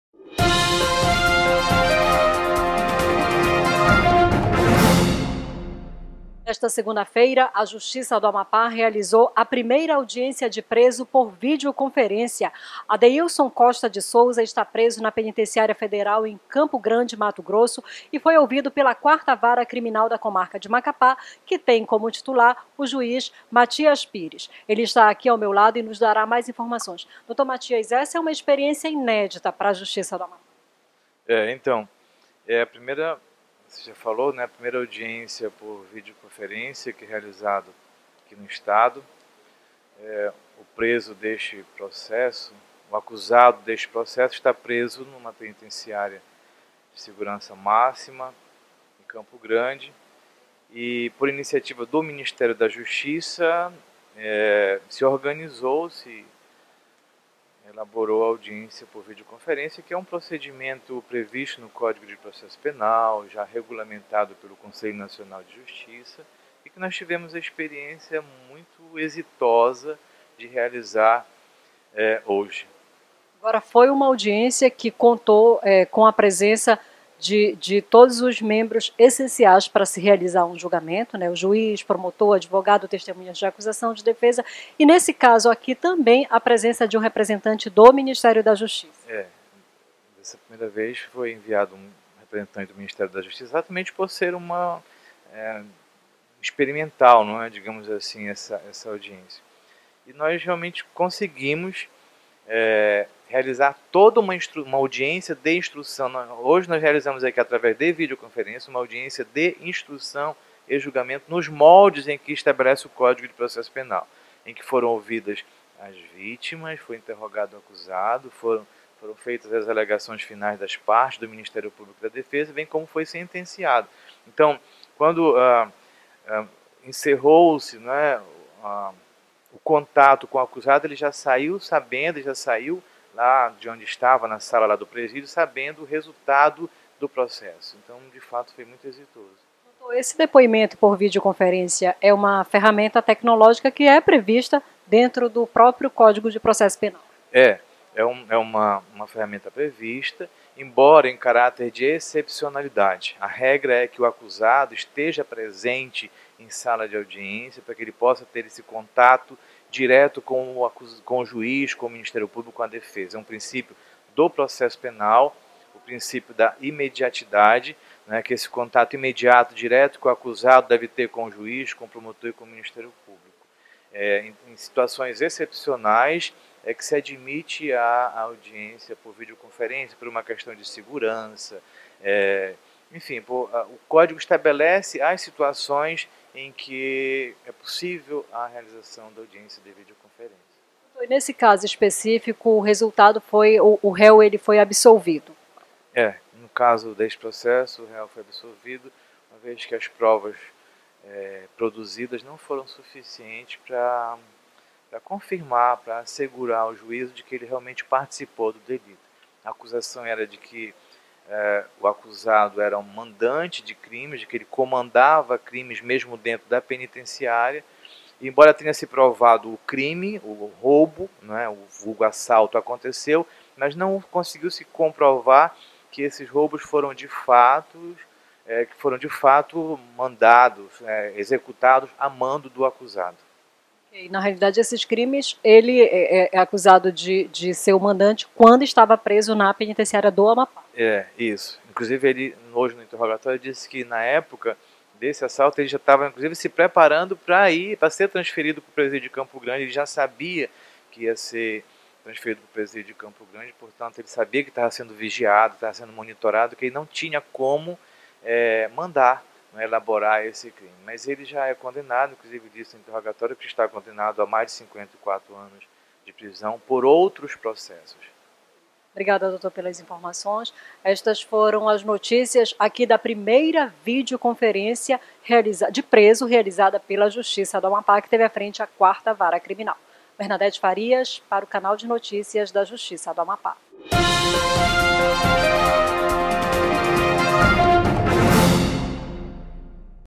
Justiça do Amapá realizou a 1ª Audiência de Preso por Videoconferência
Nesta segunda-feira, na sede do Tribunal de Justiça do Amapá ocorreu a primeira audiência de preso por meio de videoconferência. Adeilson Costa de Souza está preso na Penitenciária Federal em Campo Grande/MS e foi ouvido pela 4ª Vara Criminal da Comarca de Macapá, que tem como titular o Juiz Matias Pires Neto.  No Amapá ele é acusado dos crimes de assalto a mão armada e formação de quadrilha.